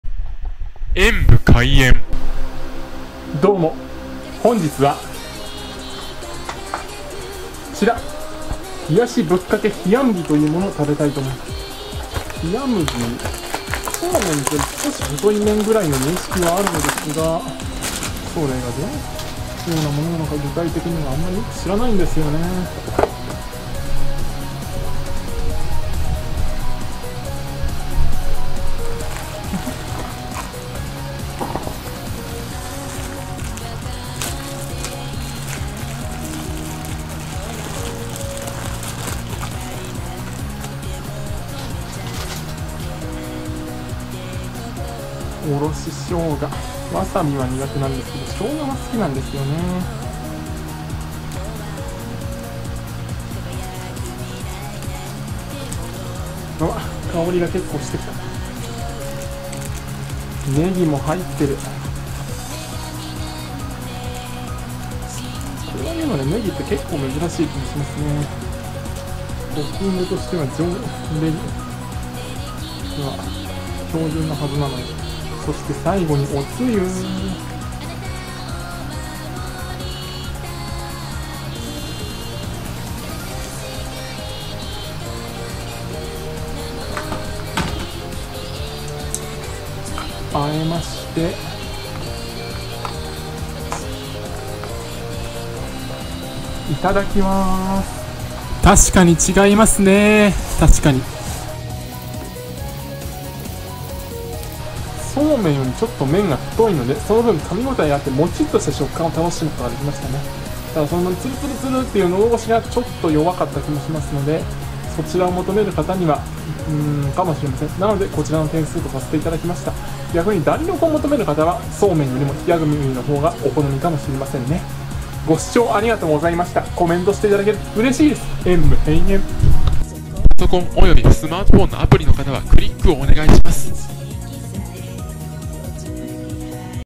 冷やし冷や麦を食べてみた。素麺とはまた違いますね！！
冷やし冷や麦を食べてみました。素麺とは似ているようで違う～
特にあそこが違いますね！！
贈り物は此方まで↓
〒153-0042